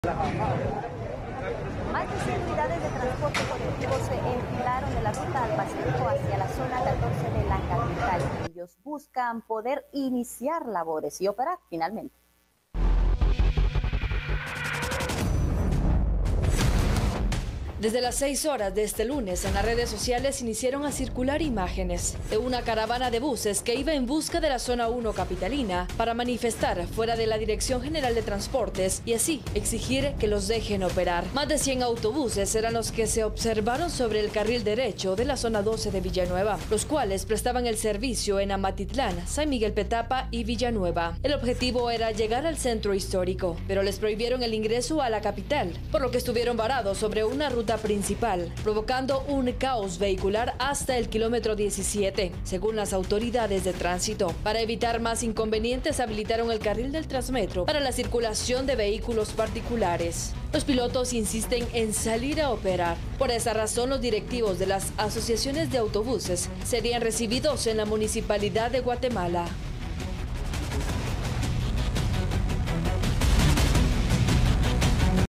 Caravana de autobuses se dirige a la capital
Busca poder iniciar sus labores
Las imágenes de la larga fila de autobuses en la ruta al Pacífico, comenzó a circular desde primeras horas de este lunes. Con rumbo a la zona uno capitalina, se movilizan con el fin de exigir a las autoridades de tránsito y transporte que les dejen operar.